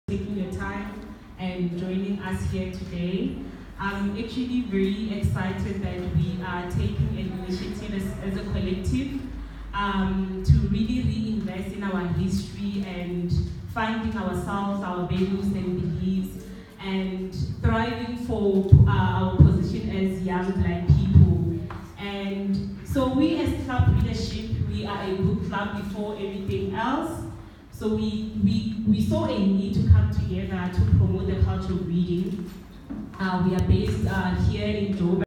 CR Book Session on Black Consciousness
Opening by Morongwa Maifo(CEO) of CR, Welcome by the manager at African Flavour Book store and take over by our facilitator of the day, a very black conscious leader KK